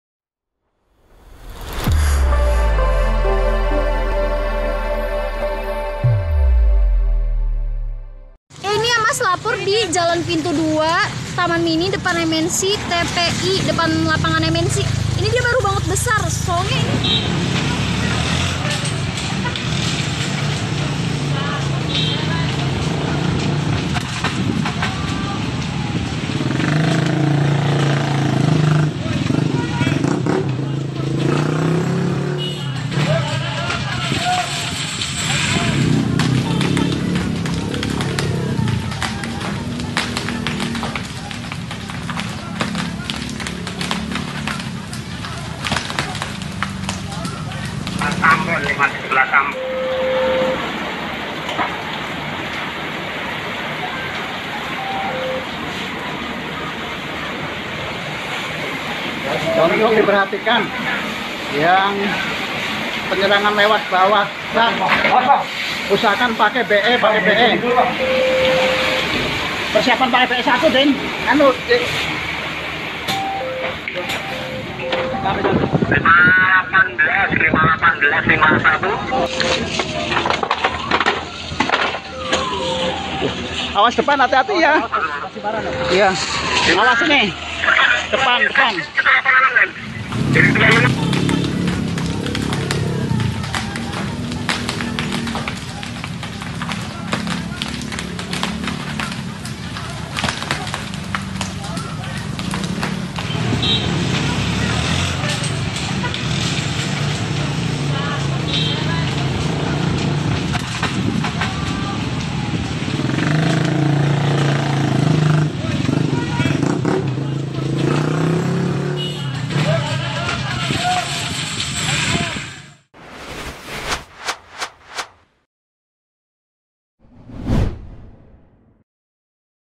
BANGUNAN RUMAH DI PINANG RANTI HANGUS TERBAKAR
Kebakaran Bangunan Rumah di Kel. Pinang Ranti, Kec. Makasar, Jakarta Timur pada Minggu (11/7/2021).

Lokasi kebakaran yang terjadi di Jl. Pintu II TMII No. 71 itu diperkirakan sekitar pada pukul 13.50 WIB.

Diduga api berasal dari konsleting listrik lantai bawah yang timbulkan api yang merambat hingga membesar pada lantai atas rumah.

Pengerahan unit sebanyak 10 unit pompa dan pendukung beserta 50 orang personil. Dalam Peristiwa ini tidak ada korban jiwa, namun kerugian ditaksir hingga ratusan juta rupiah.